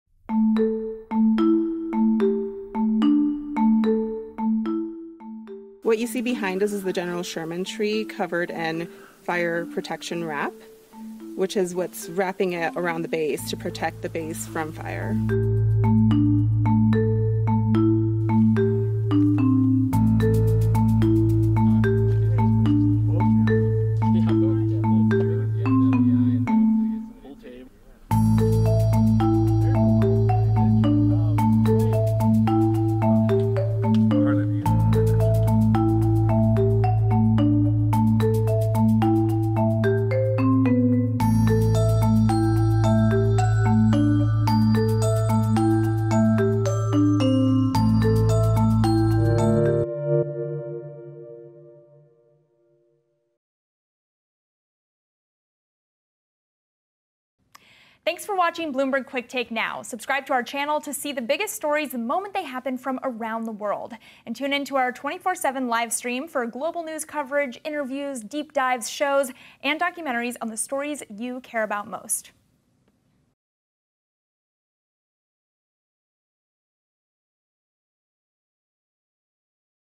Why Is California Wrapping Giant Seqoia Trees in Silver Foil?
Fire crews in Sequoia National Park in California's Sierra Nevada are covering the base of some of the park's famous giant trees with fire protection wrap.

The wrap is meant to help guard the trees as more than 1,700 firefighters battled the KNP Complex Fires.

That fire complex covered 76 square miles (196 square kilometers) by Wednesday.

National Park Service spokeswoman

Sintia Kawasaki-Yee said the General Sherman Tree, the world's largest tree, is among those with the protective covering.

National Park Service Fire Information Officer Mike Theune said fire came very close to the massive tree less than two weeks ago.

Theune said the park service has conducted prescribed burns in Sequoia National Park for more than 50 years to reduce the vulnerability of the massive trees.

He also said the giant Sequoias live for centuries and have survived many fires over their long history.